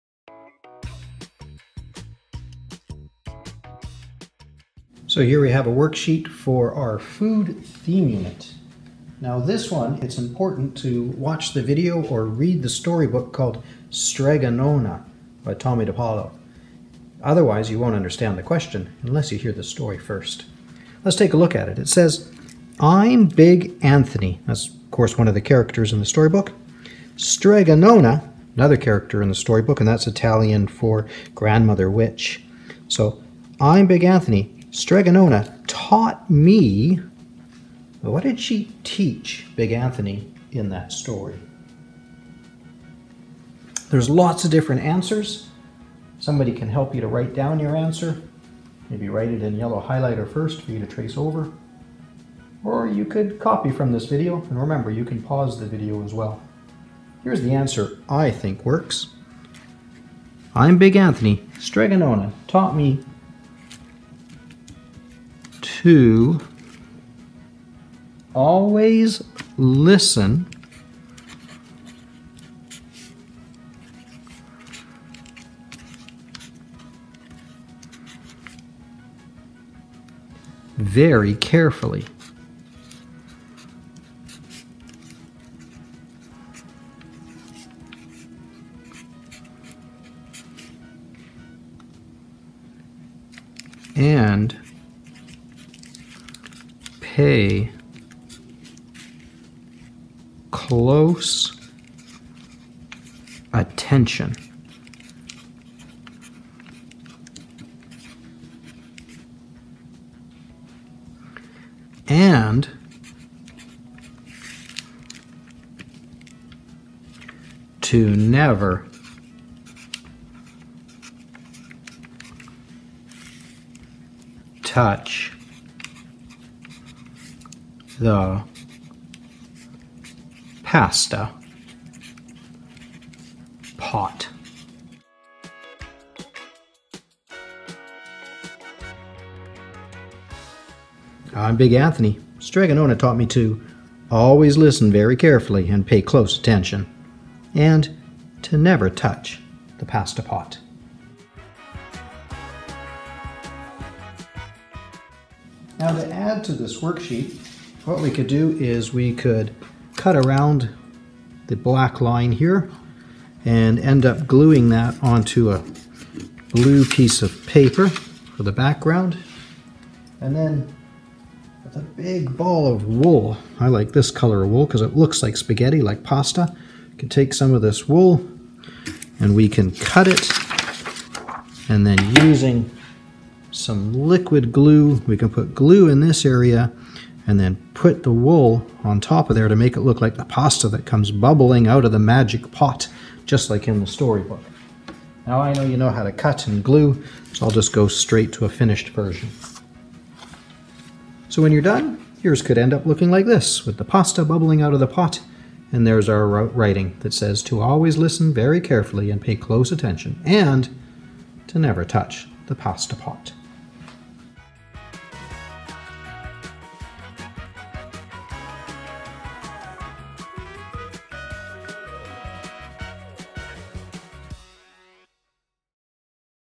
Big Anthony Pasta Pot
Mr. C. walks through a worksheet thaat goes with the storybook Strega Nona. What did Big Anthony learn from her. Cut and paste.